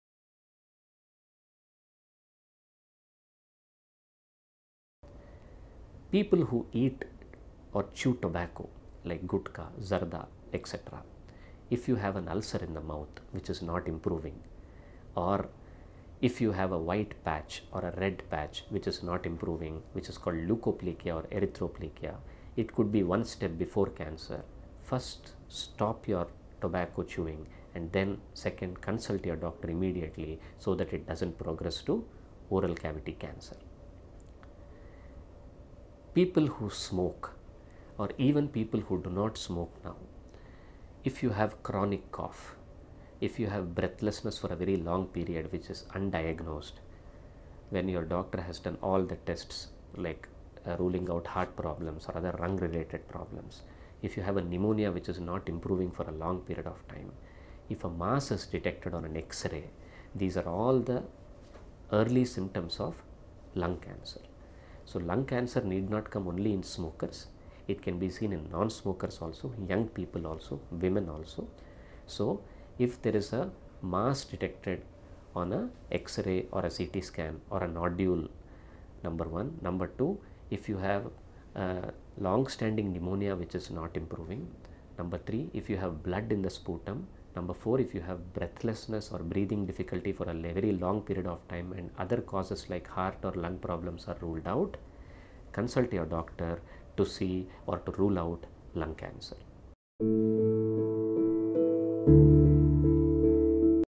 What are the ill effects of tobacco consumption? Dr. Krishnamani.
Dr. Krishnamani highlights the detrimental impact of tobacco consumption on individuals and society.

Tobacco use in any form, whether smoking or smokeless, poses significant health risks that can lead to life-threatening conditions.

Smokeless tobacco can cause gum recession, tooth decay, and oral lesions, significantly impacting oral health.

Increased Heart Risks: Smokeless tobacco products contain nicotine, contributing to increased heart rate and blood pressure.

Cancer Risk: Chewing tobacco is linked to cancers of the mouth, tongue, oesophagus, and pancreas.

Secondhand Smoke: Non-smokers exposed to secondhand smoke also face health risks, including an increased likelihood of respiratory illnesses, heart diseases, and lung cancer.

Quitting tobacco is the most beneficial decision one can make for their health and the well-being of loved ones.

Let's unite to combat the menace of tobacco, empower individuals to lead healthier lives, and create a tobacco-free future for generations to come!